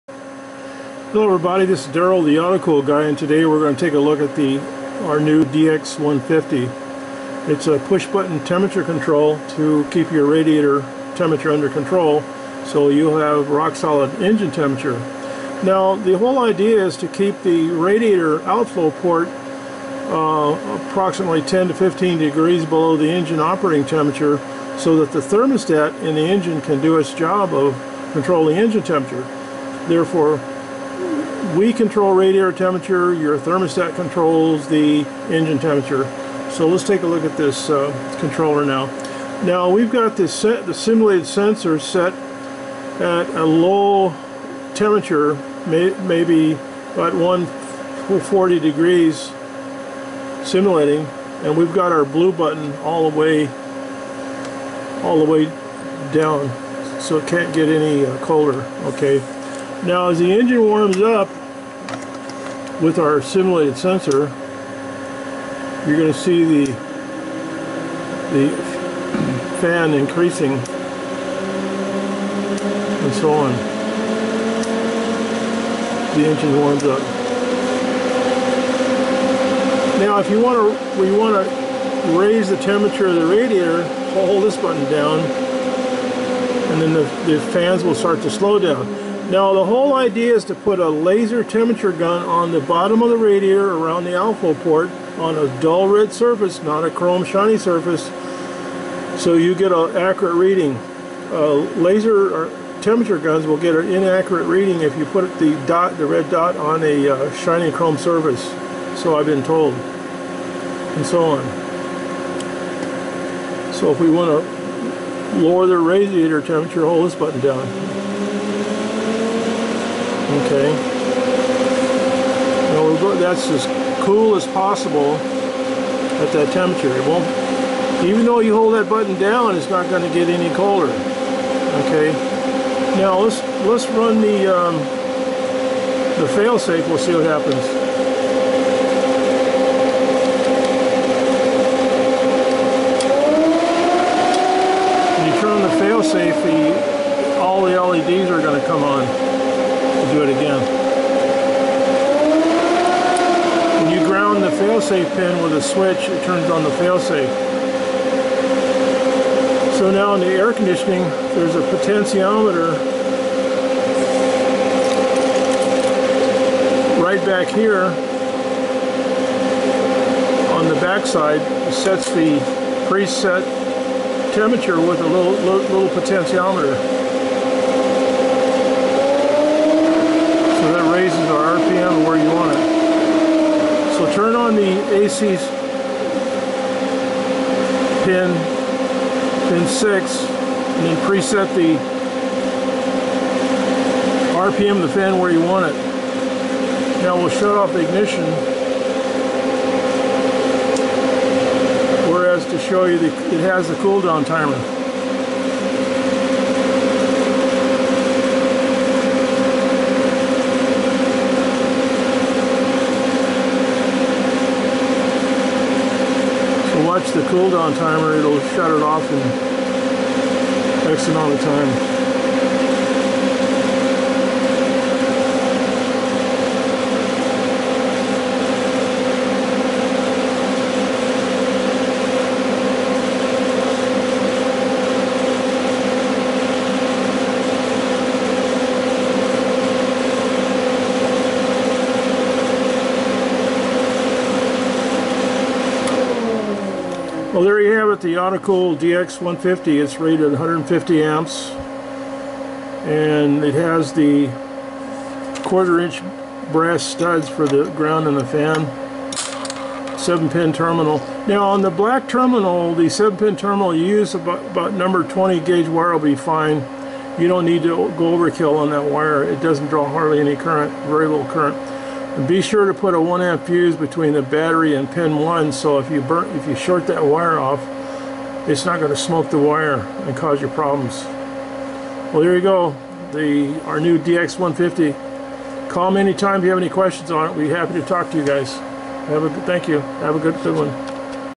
DX-150 RADIATOR FAN CONTROLLER - THE AUTO COOL GUY
The Auto Cool Guy has several PWM, Pulse Width Modulated Radiator fan temperature controllers.